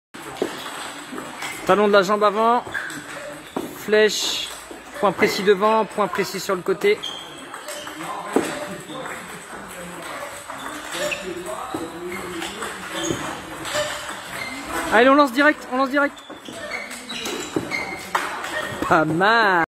Ouf Alexis LEBRUN est bien meilleur au Ping-Pong qu'aux fléchettes 🎯😁
Dans cette vidéo datant d'avril 2021, Alexis Travaille la préparation perceptive associée à un travail vestibulaire en situation de déséquilibre 🎯.

Bon, pour résumer, Alexis est bien meilleur au Ping qu'aux fléchettes 😁

Merci à Jérémy Surault pour la vidéo